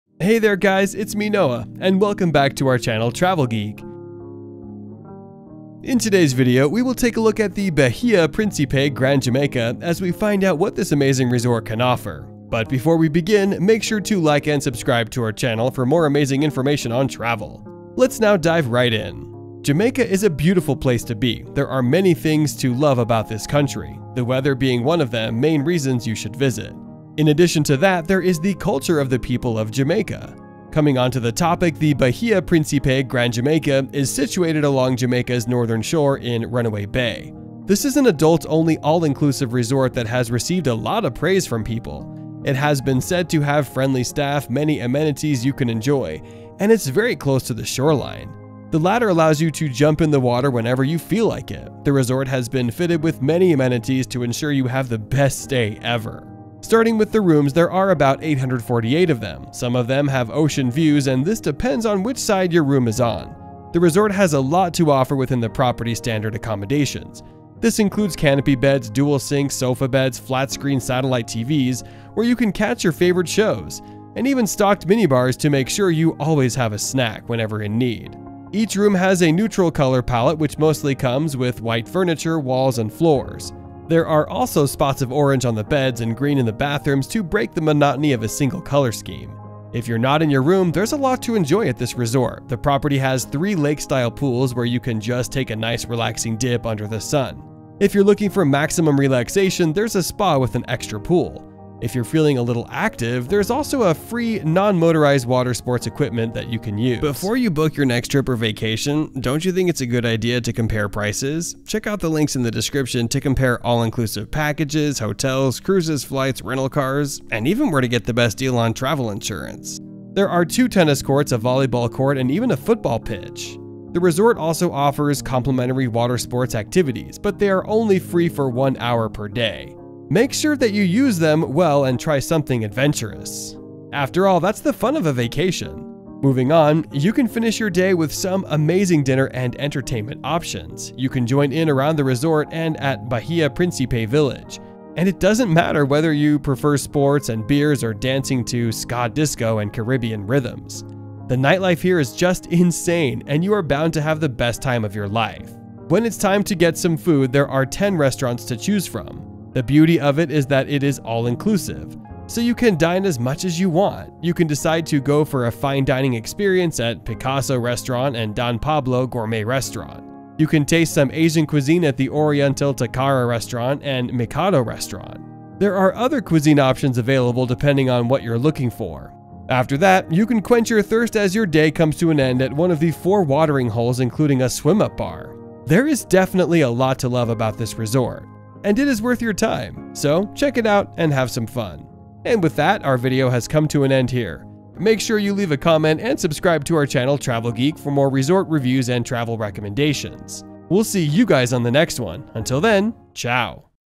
Bahia Principe Grand Jamaica Hotel Tour & Review | Jamaica All Inclusive Resorts
If you have any concerns about content being shared in this video or would like credit, please do not hesitate to reach out to .
Bahia Principe Grand Jamaica

The Bahia Principe Grand Jamaica is situated along Jamaica’s northern shore in Runaway Bay. This is an adults-only, all-inclusive resort that has received a lot of praise from people. It has been said to have friendly staff, many amenities you can enjoy, and it is very close to the shoreline. The latter allows you to jump in the water whenever you feel like it. The resort has been fitted with many amenities to ensure you have the best stay ever.